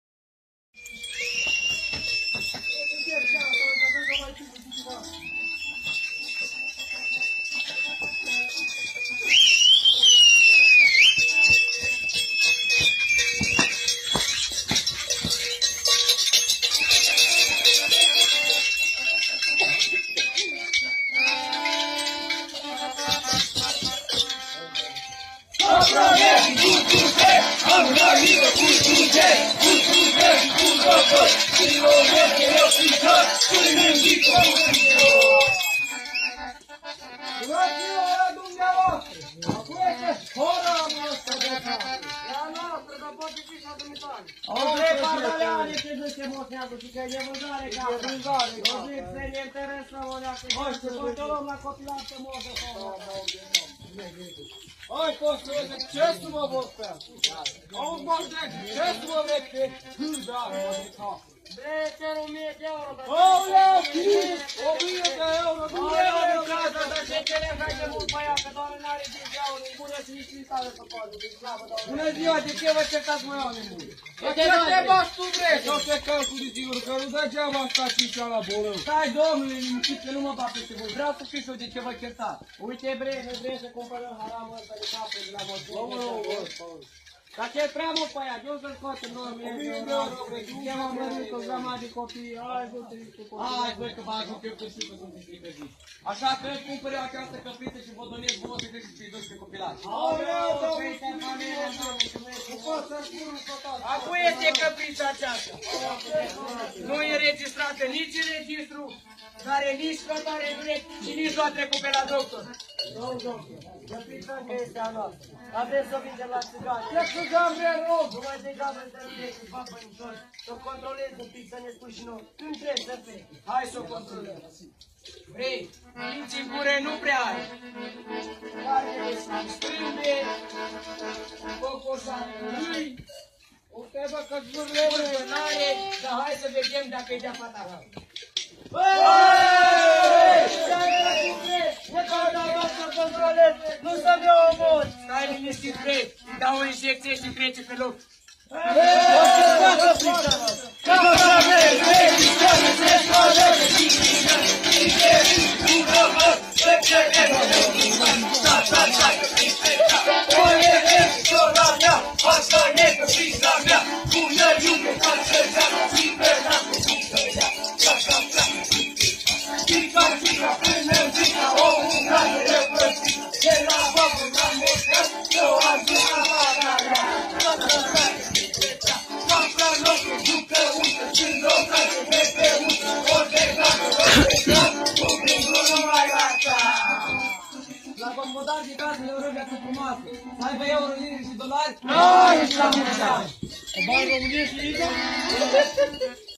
"Capra" la Vicovu de Jos (2017)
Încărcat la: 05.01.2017
Titlu original: Capra din Vicovu de Jos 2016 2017 by Ionuț Kr
Performeri: tineri din sat